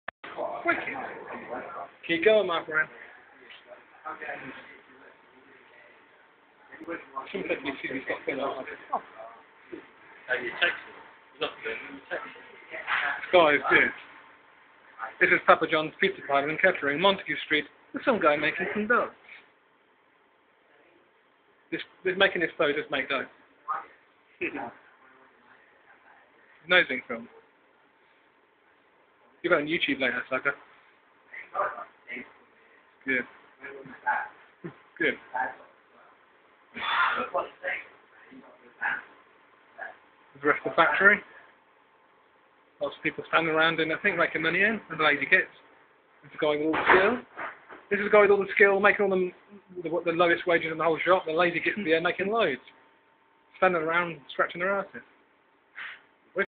Papa John's, creating the best pizzas EVER!
Local branch of "Papa John's" knocking up some delicious pizzas! best ever!